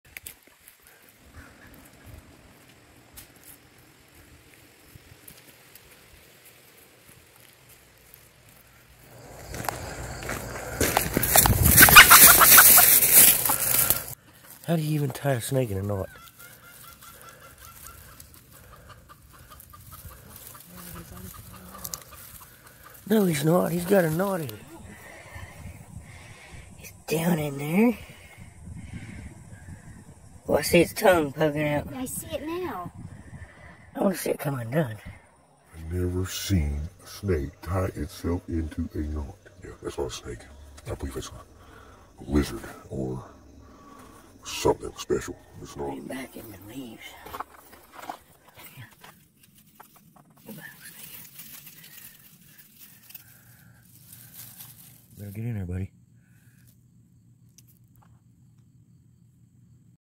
RARE Hoop Snake Saved From Chicken
At first glance, I thought my chicken had a toad or frog but when I caught it I discovered what I believe to be a legless lizard not a snake. although in my experience a glass lizard will break apart which this one did not.. If anyone knows what type of eastern U.S. creature ties itself into a knot and then unties itself easily please leave your answer in the comments.